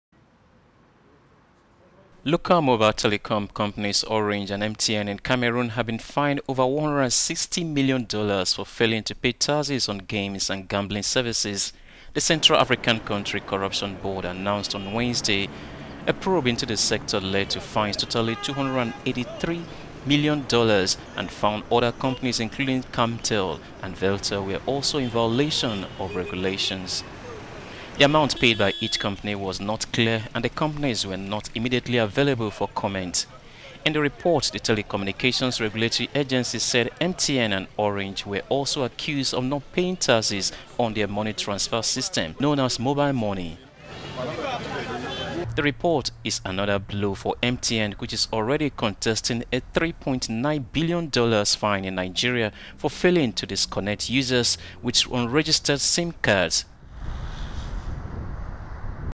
Cameroon fines main local telecom companies
The local mobile telecom companies Orange and MTN in Cameroon have been fined over $160 million for failing to pay taxes on games and gambling services.

The Central African country corruption board announced on Wednesday.

A probe into the sector led to fines totaling $283 million, and found other companies including Camtel and Viettel were also in violation of regulations.

The amount paid by each company was not clear and the companies were not immediately available for comment.